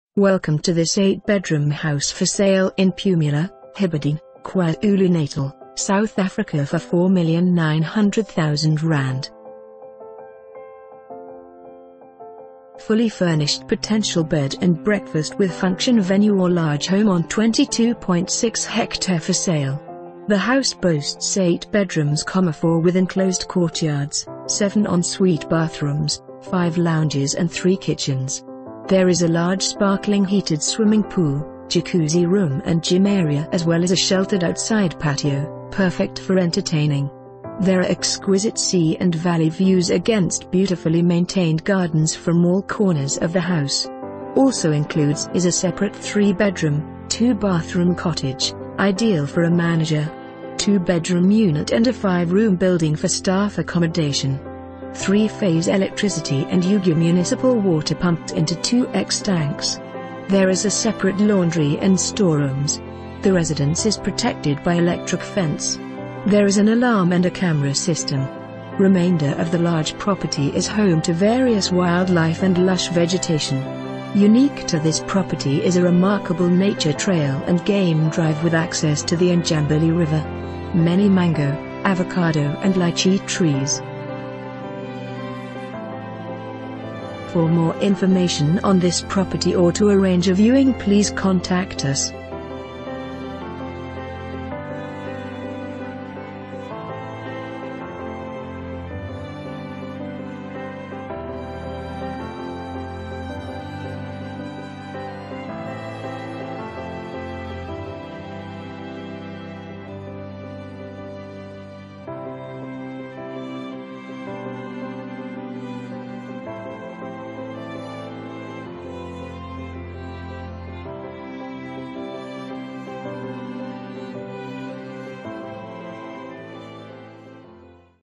8 bedroom House For Sale in Pumula, Hibberdene, KwaZulu Natal for ZAR 4,900,000
Fully Furnished Potential Bed and Breakfast with Function Venue or Large Home on 22.6 ha for sale

The house boasts 8 bedrooms ,4 with enclosed courtyards, 7 en-suite bathrooms, 5 lounges and 3 kitchens.

There is a large sparkling heated swimming pool, Jacuzzi room and gym area as well as a sheltered outside patio - perfe...